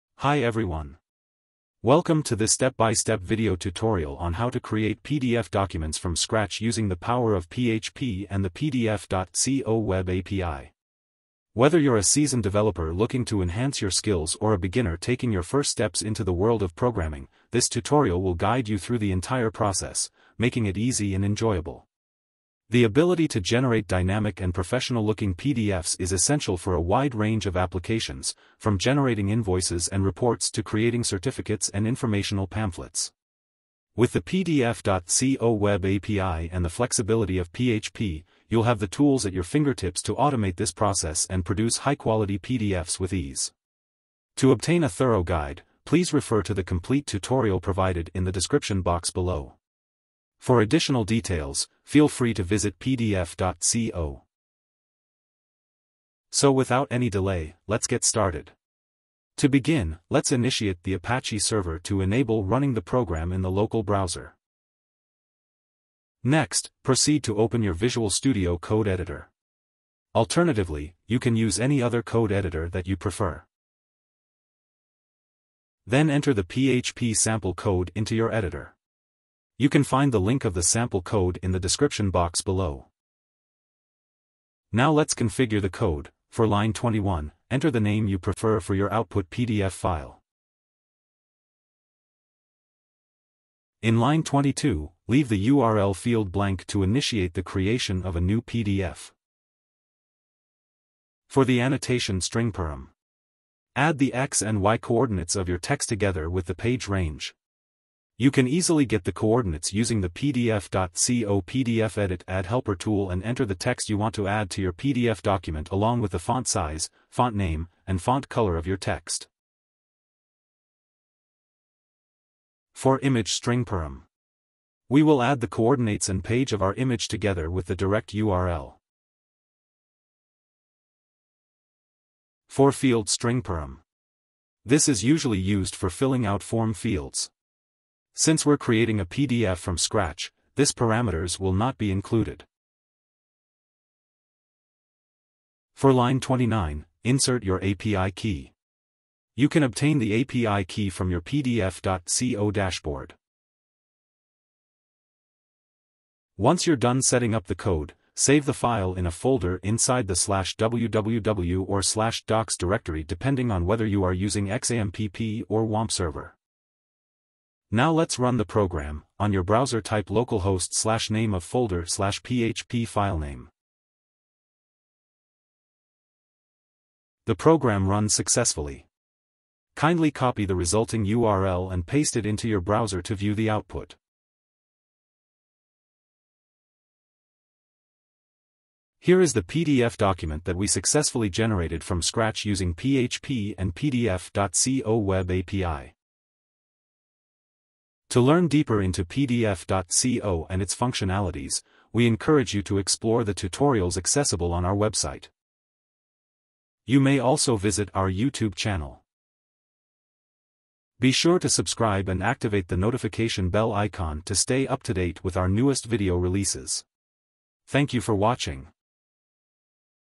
How to Create PDF from Scratch in PHP using PDF.co Web API
This video will demonstrate creating PDFs from scratch in PHP using PDF.co Web API.

A captivating journey into the world of PDF creation, where you'll learn the art of crafting PDFs from scratch. Involve yourself in the remarkable capabilities of the PDF.co Web API within the context of PHP through this all-comprehensive tutorial. Uncover the secrets behind the creation of dynamic, customized PDF documents.

Throughout this tutorial, you'll learn deeply about the core concepts and techniques involved in building PDFs from scratch. We'll explore a range of topics, from document structure and content formatting to incorporating graphical elements and interactive features. By the time you reach the end of this journey, you'll possess the expertise to design visually appealing PDFs enriched with advanced functionalities.

Furthermore, practical examples will guide you through a hands-on learning experience. You'll have the opportunity to experiment with different approaches and modify them to meet your specific needs. This tutorial includes a variety from fundamental PDF generation to more advanced techniques, enabling you to confidently produce professional-grade PDFs with ease.

Whether you're a seasoned PHP developer looking to enhance your document generation capabilities or a newcomer excited to explore the world of PDF creation, this tutorial is your gateway to success. Release your creativity, elevate your projects, and become a master in the art of crafting PDFs from scratch using the PDF.co Web API in PHP. Prepare to initiate this enriching journey and unlock a field of possibilities in PDF generation.

In summary, this tutorial has equipped you with a comprehensive grasp of generating PDFs from scratch using the PDF.co Web API in PHP. You now possess essential knowledge of concepts, techniques, and best practices for fashioning personalized and dynamic PDF documents.